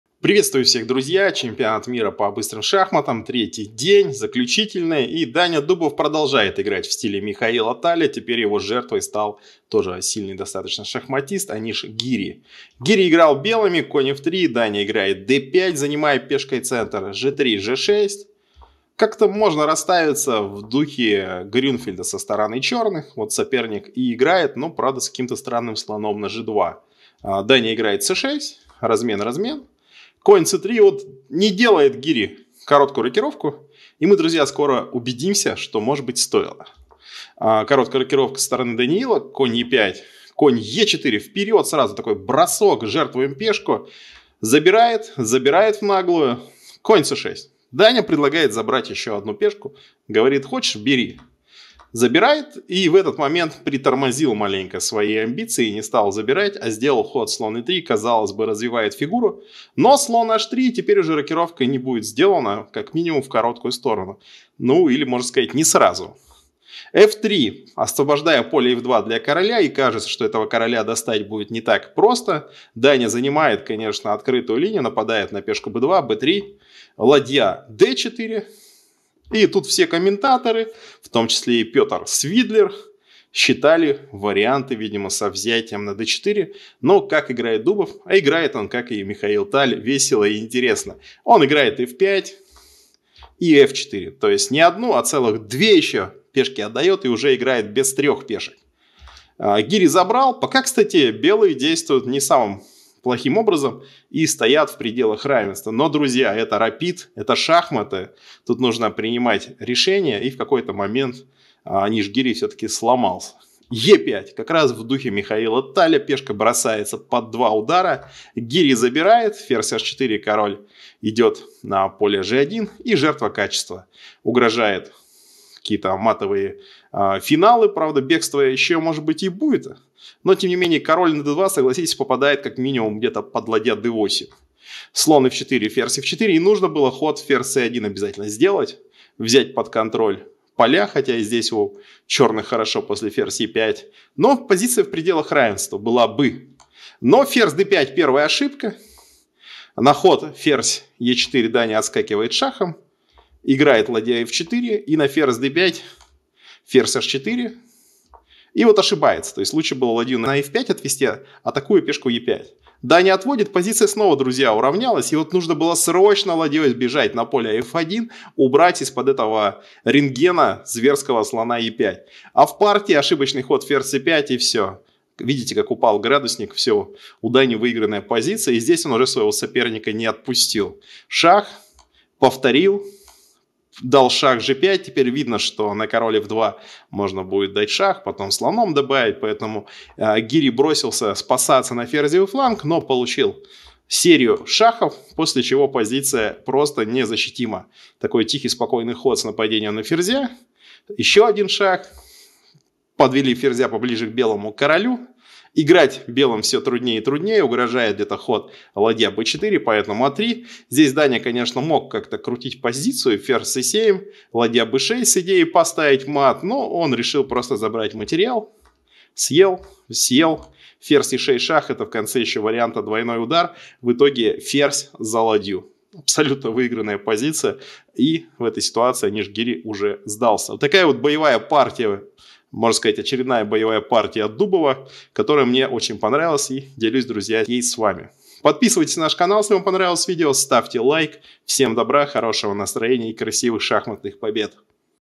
Яростная атака Дубова! Разорвал Гири уже в дебюте. Шахматы
Чемпионат мира по рапиду и блицу. Алма-Ата.
3 день. Аниш Гири - Даниил Дубов. 0-1.
Дата проведения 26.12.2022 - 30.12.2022. Швейцарская система.
Общий призовой фонд – 1 миллион  долларов. Рапид и блиц: 180 участников, 102 участницы.
Контроль времени: рапид – 15 минут + 10 секунд на ход, блиц – 3 минуты + 2 секунды на ход.
Игровые дни: 26-28 декабря (рапид), 29-30 декабря (блиц).
Начало игры в 12.00, в последний день в 11.00 msk